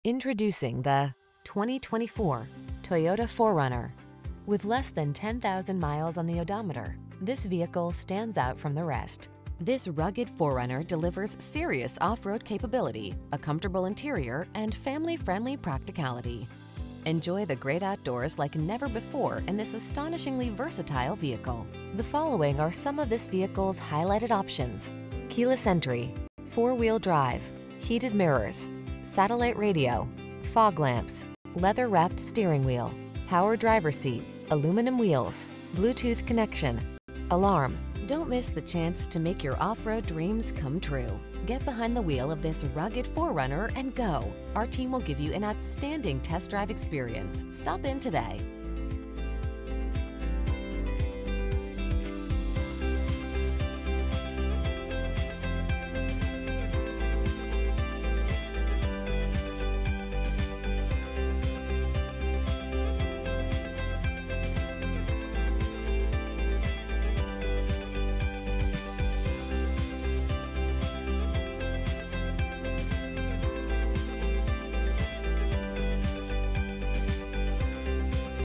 2024 Toyota 4Runner TRD Off Road UT Orem, Provo, Pleasant Grove, Vineyard, Lehi
Pre-Owned 2024 Toyota 4Runner TRD Off Road available at Orem Toyota located in 1400 Sandhill Rd, Orem, UT, 84058. Servicing the Orem, Provo, Pleasant Grove, Vineyard, Lehi area.

Introducing the   2024  Toyota 4Runner.  This rugged 4Runner delivers serious off-road capability, a comfortable interior, and family-friendly praciticality. Enjoy the great outdoors like never before in this astonishingly versatile vehicle. The following are some of this vehicles highlighted options: Keyless Entry, Four Wheel Drive, Heated Mirrors, Satellite Radio, Fog Lamps, Leather Wrapped Steering Wheel, Power Driver Seat, Aluminum Wheels, Bluetooth® Connection, Alarm. Don't miss the chance to make your off-road dreams come true. Get behind the wheel of this rugged 4Runner and go! Our team will give you an outstanding test drive experience. Stop in today!